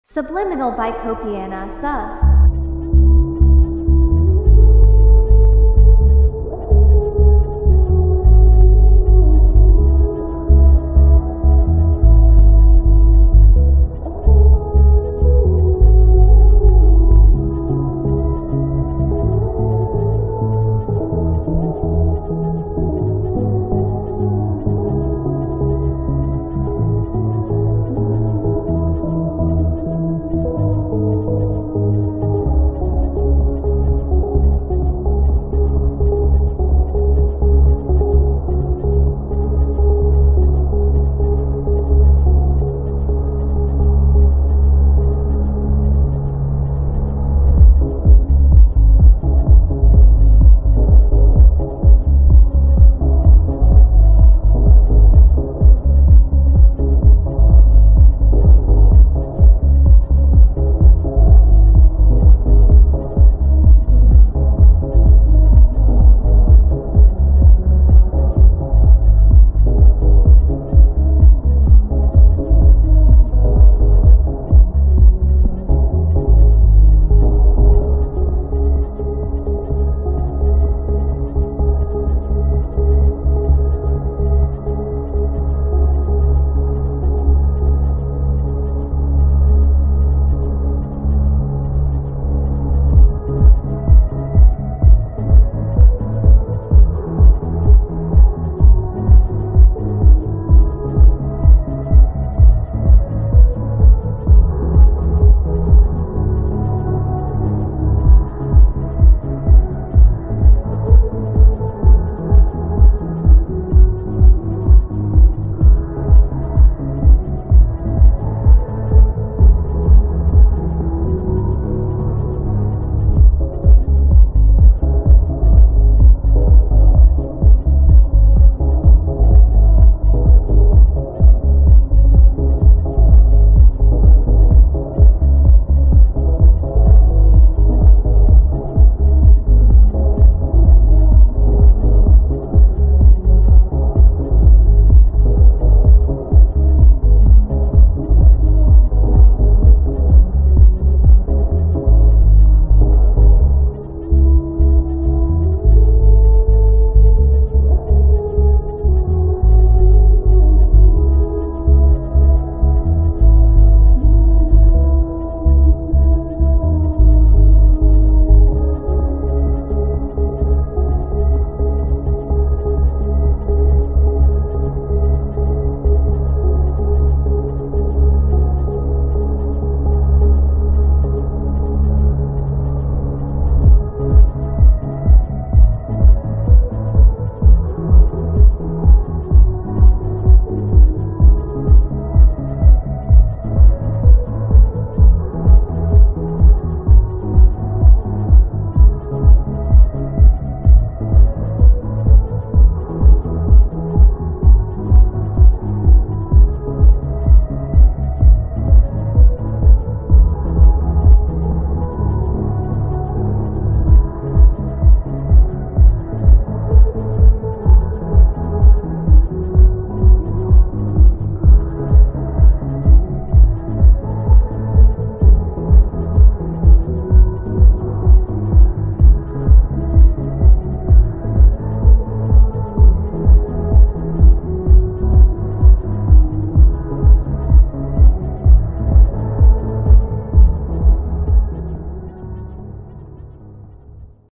flat belly subliminal ( one line formula ) | copiana subs
ONE LINE FORMULA
   - just ONE affirmation that is played repeatedly ( ex : I have a flat belly )

made this for my personal use ( experimenting it ) but I guess I'd share it with y'all

                                                     xoxo, yana